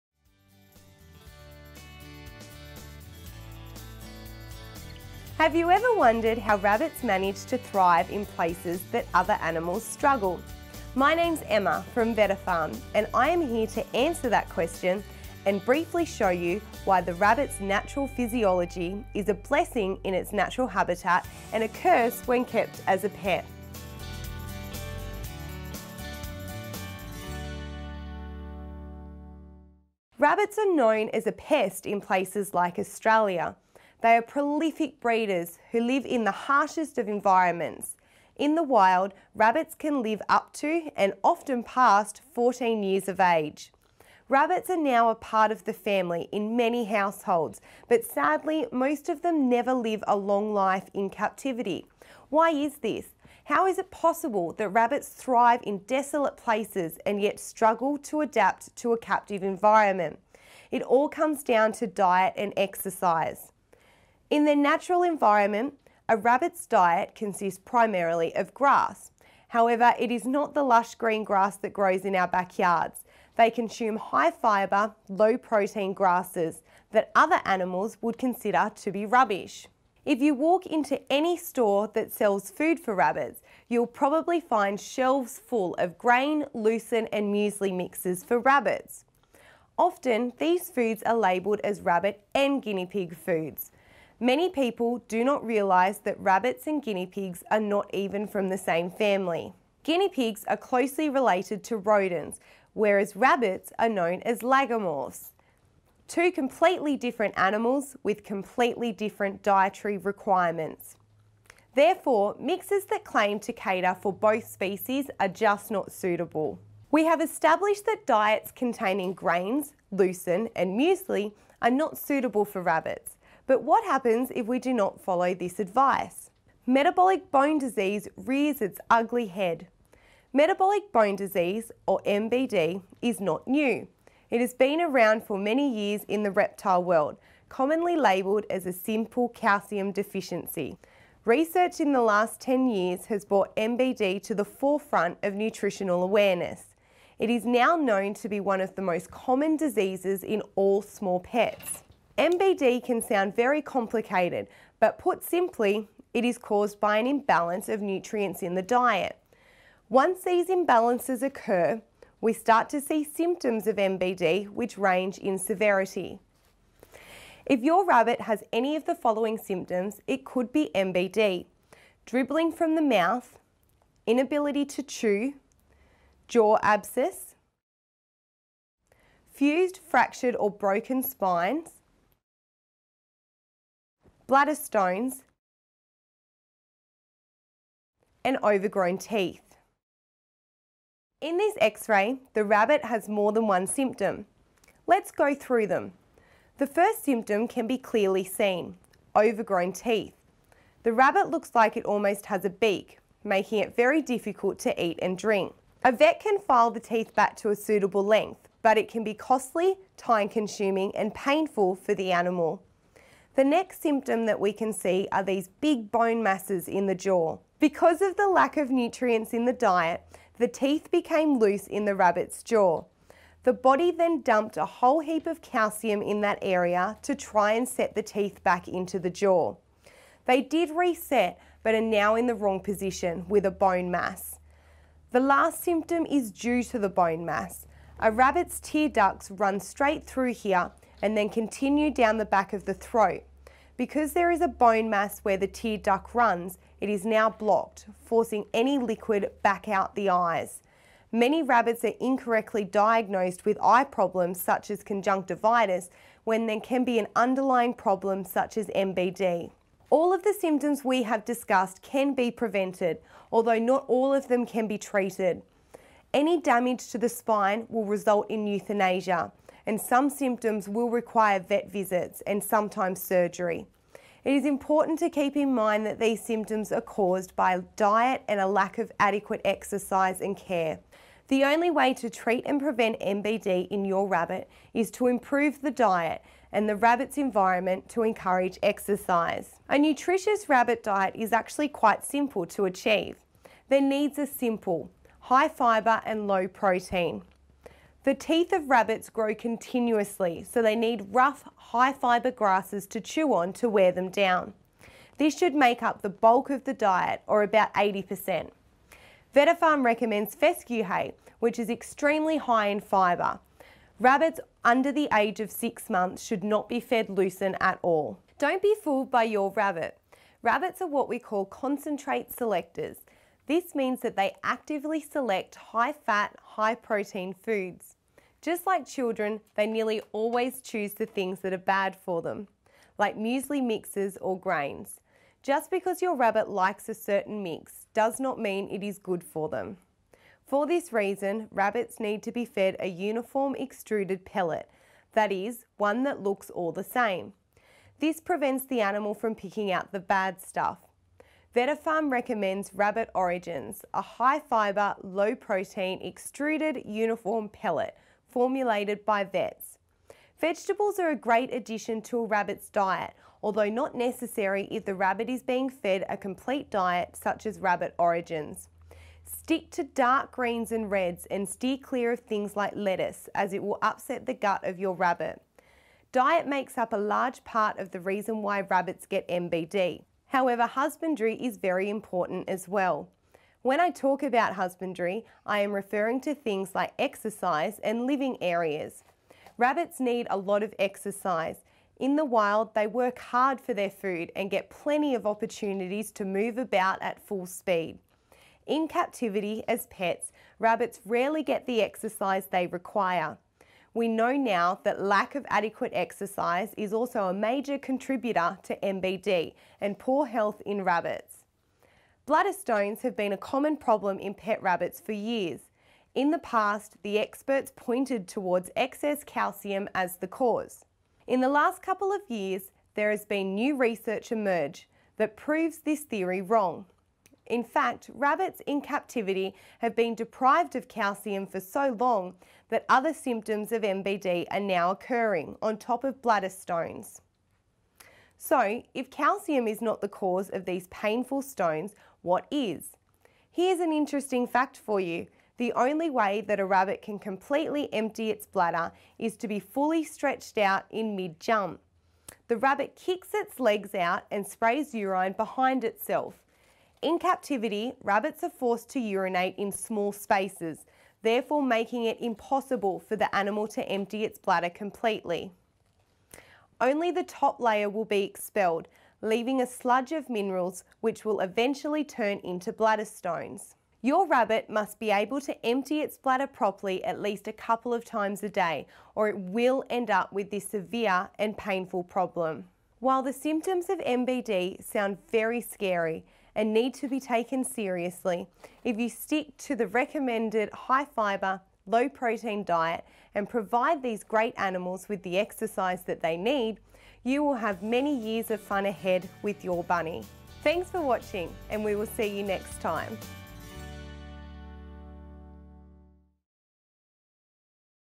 Rabbit Health - MBD (Metabolic Bone Disease)
Metabolic Bone Disease, or MBD, is one of the most common diseases in pet Rabbits. Vetafarm's Emma Harris takes us through the causes, symptoms and treatments for this life threatening illness.